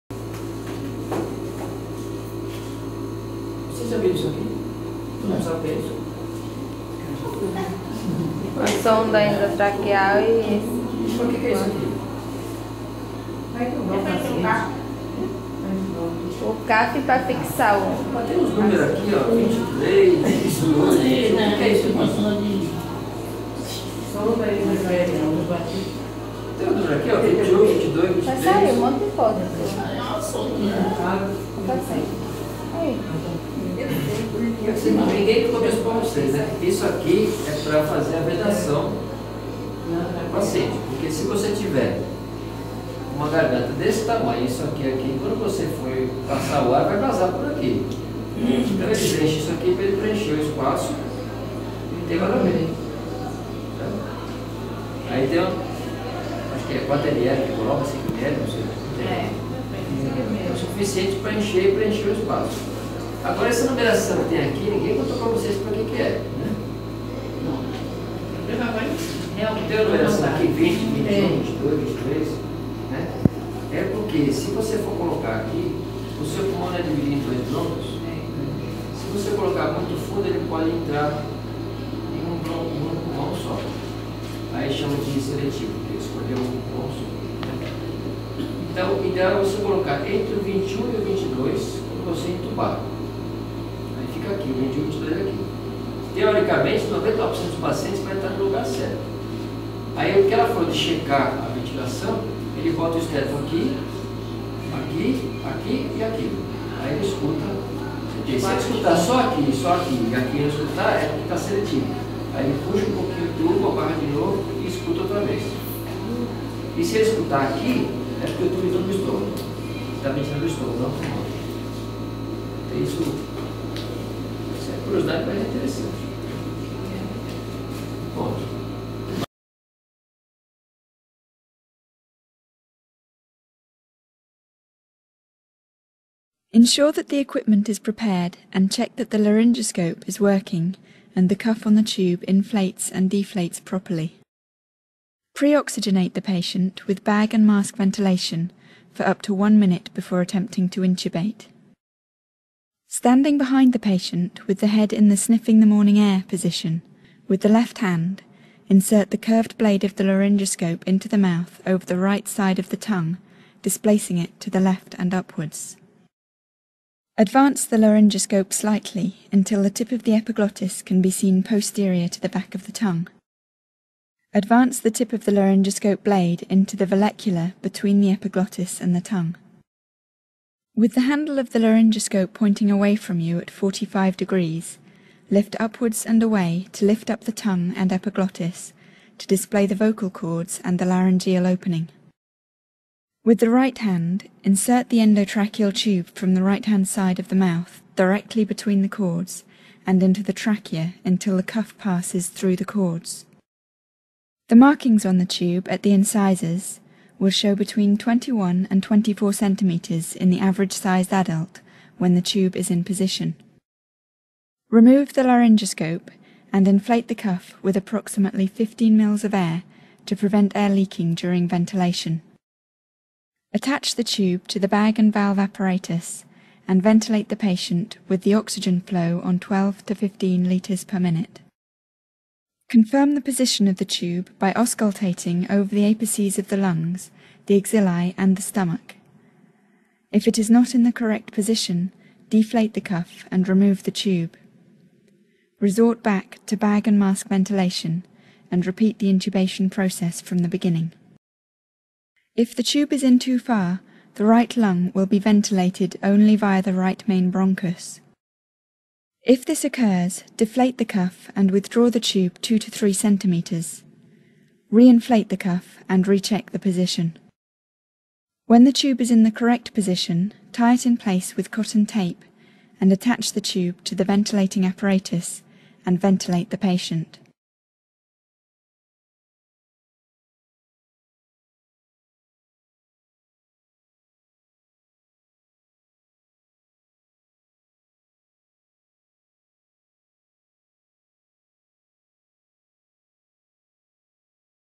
Capacitação EBEM HEC Tubo Endotraqueal
Parte da Capacitação ministrada no Hospital Ermírio Coutinho em Nazaré da Mata
Considerações sobre Tudo Endotraqueal e Intubação.